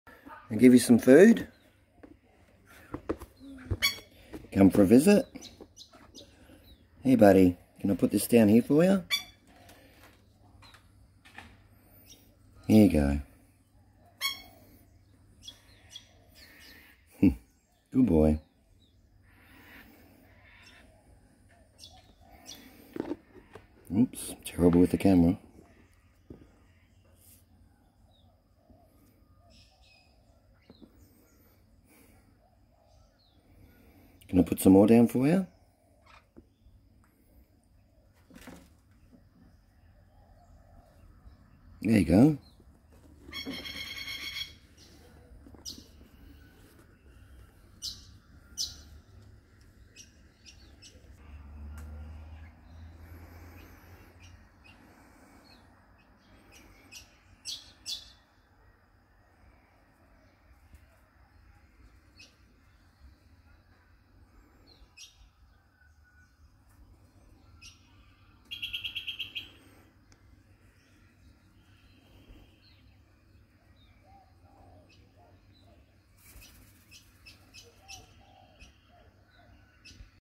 Red-winged Parrots
These Red-winged parrots don't visit all the time.  Usually about once every 3 weeks or so.